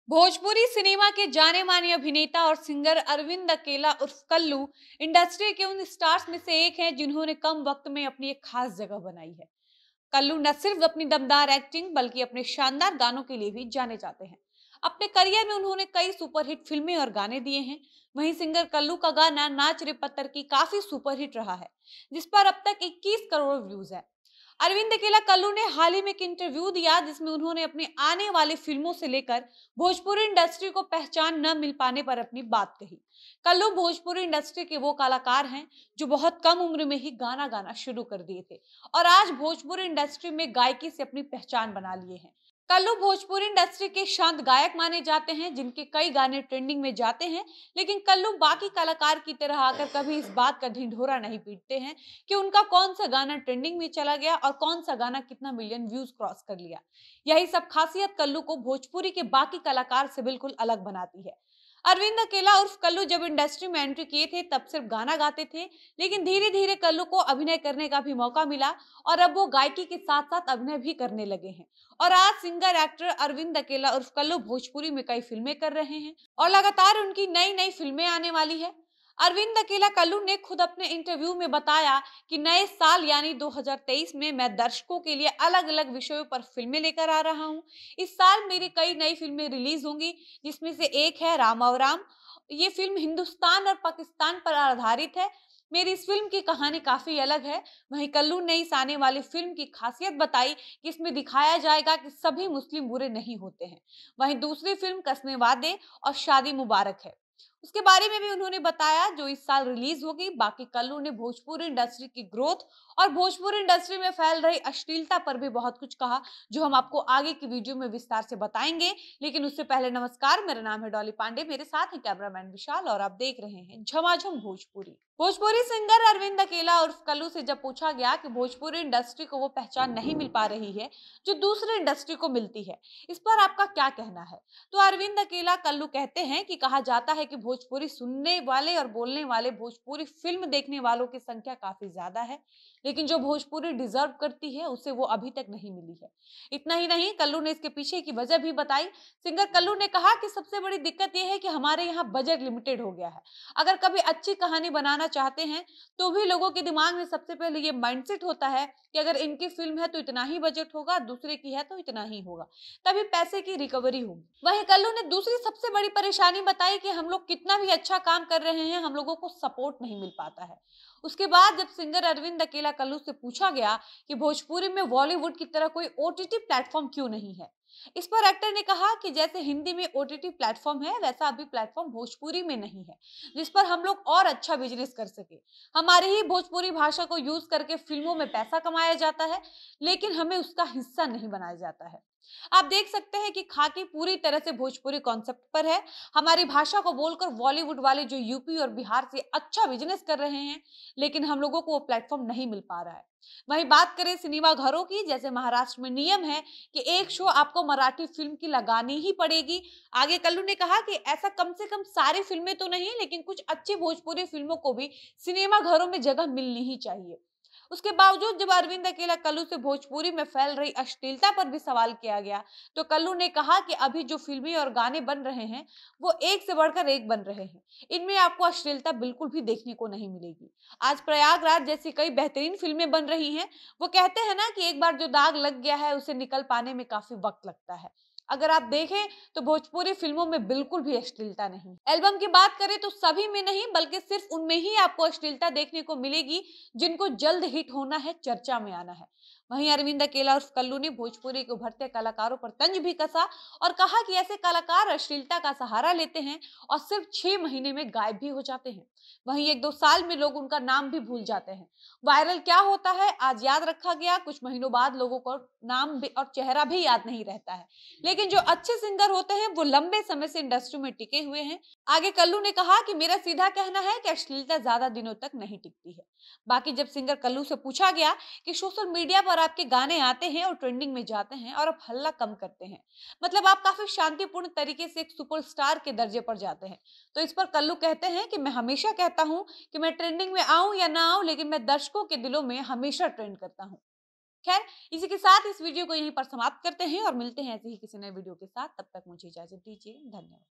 Kallu ने Bhojpuri में अश्लीलता पर खुल कर दिया जवाब, अश्लील गाने वाले को 6 महिने बाद कोई नहीं जानता!
गायक अरविन्द अकेला कल्लू ने भोजपुरी इंडस्ट्री में गंदा गाने वालों पर साधा निशाना कहा, सब के सब फुस्स हो जायेंगे कोई नहीं बचेगा! साथ ही अपनी आने वाली फिल्मों के बारे में भी बताया, जानिए पुरी बात!